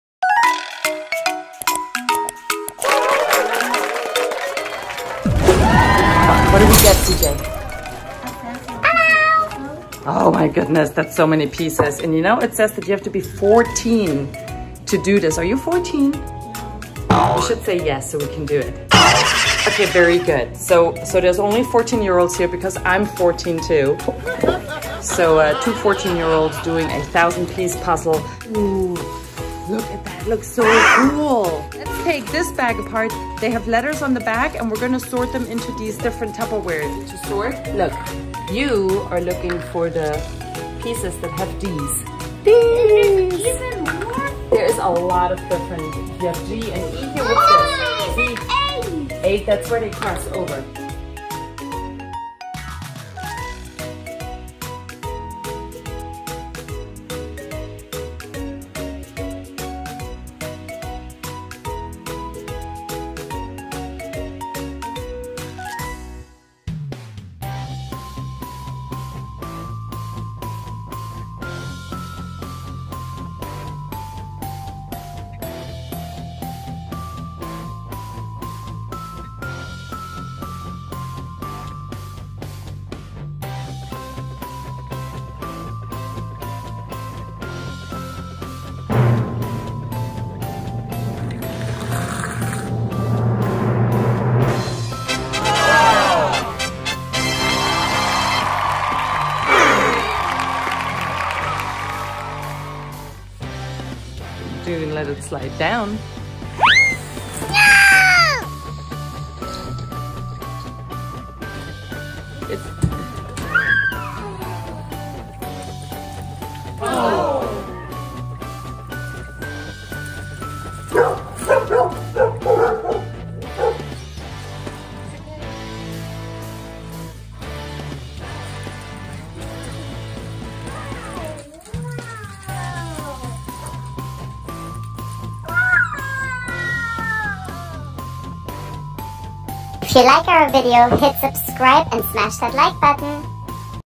1000 piece puzzle DESTROYED! | Kidsstories | puzzle Unboxing | The CJ & LJ Show
Thank you Eaki Amazon Store for this supercool 1000-piece puzzle of Animals in the Jungle!
CJ, LJ and Mommy had so much fun creating this awesome puzzle and then CJ got to destroy it!!!

This puzzle has letters printed on the back  (A through J) and we sorted the puzzle pieces according to letters first creating section which we puzzled one after the other. This way we were able to puzzle sections of the puzzle one after the other and were able to complete it within one week puzzling every evening for about one hour.

If you are contemplating getting by this for your child: Mommy ended up finishing most of the sorting and puzzling and CJ helped a little here and there. So unless your kid LOVES puzzling make sure you enjoy puzzles yourself 😂.

Stay happy and keep having fun Friends and Supporters!

CJ, LJ and Mommy